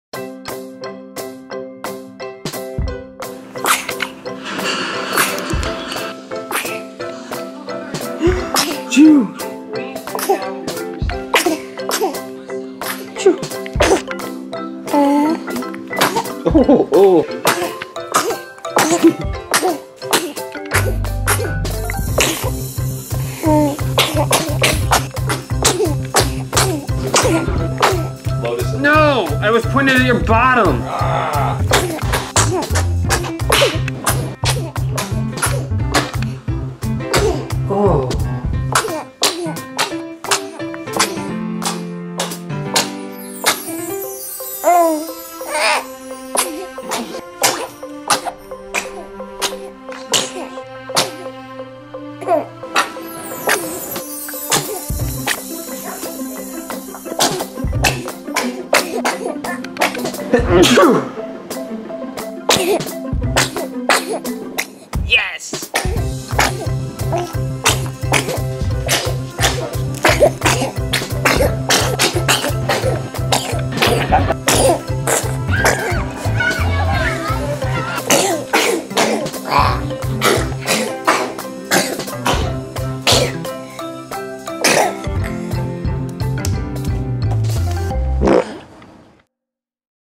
Baby Sneezes 100 times in 90 Seconds!
If one baby sneeze is cute, then a hundred will probably kill you. Watch baby Will "A-choo!" his way through life in a most adorable fashion.